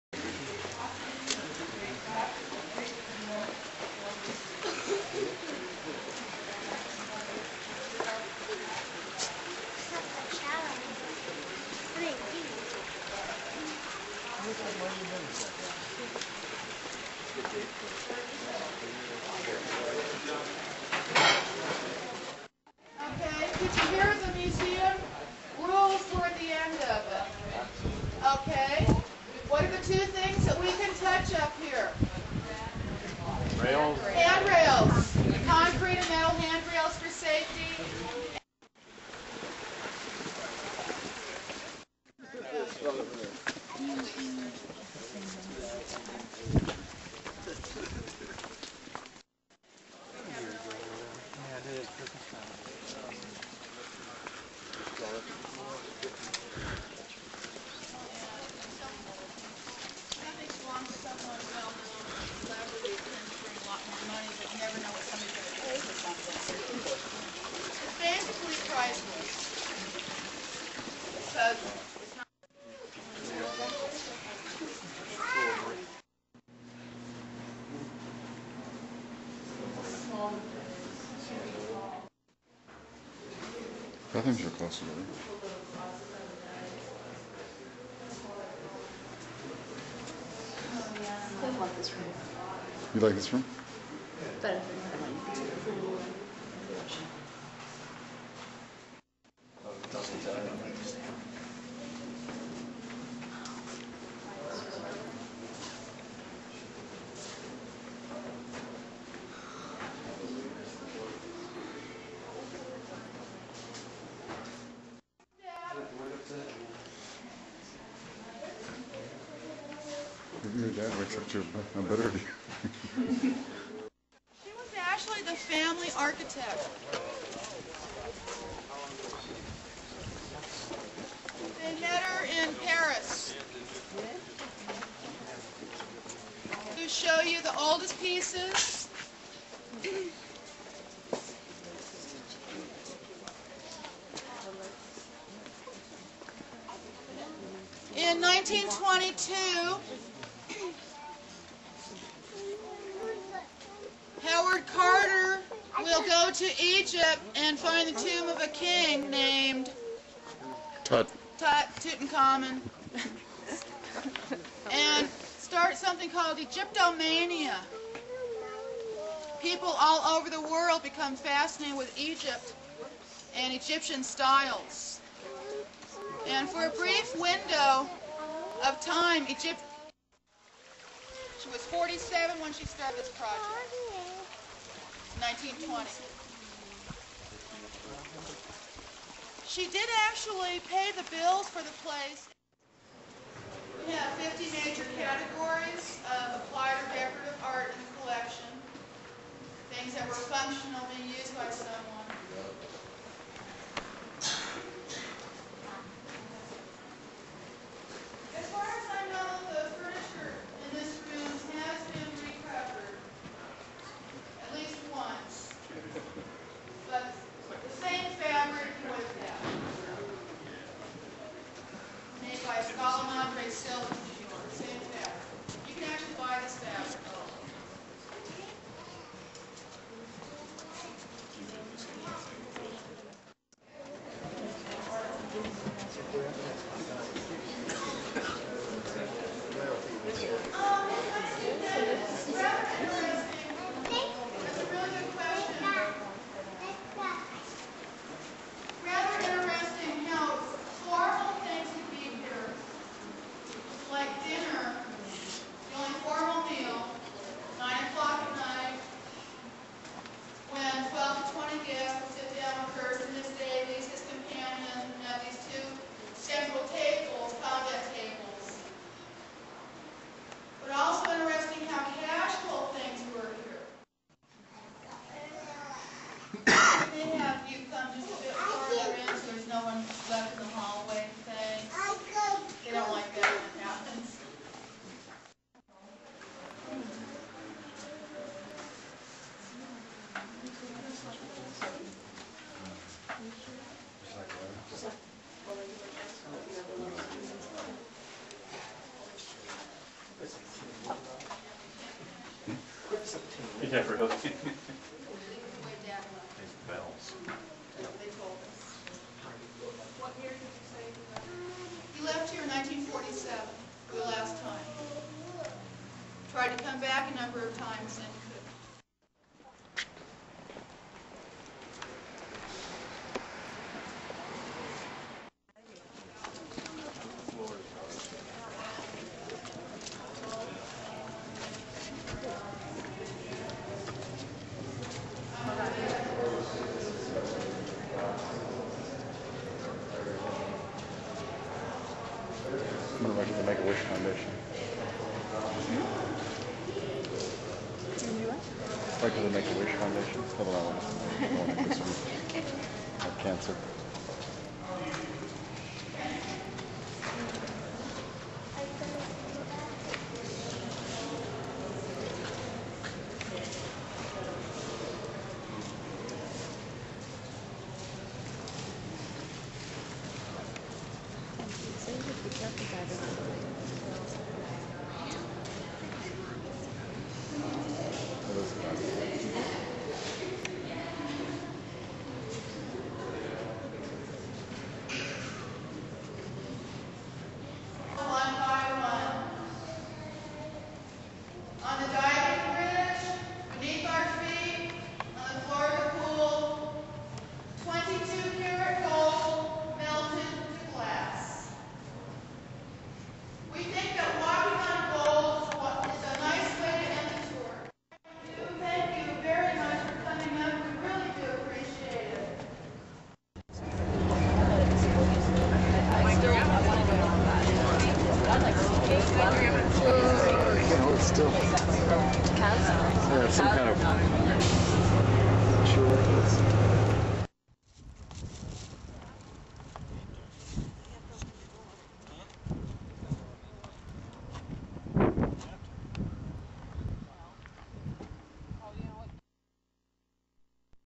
Hearst Castle
Mr. Mendoza visits the Hearst Castle in San Simeon, CA. This is the former home of Yellow Journalist, Newspaper Tycoon William Randolph Hearst.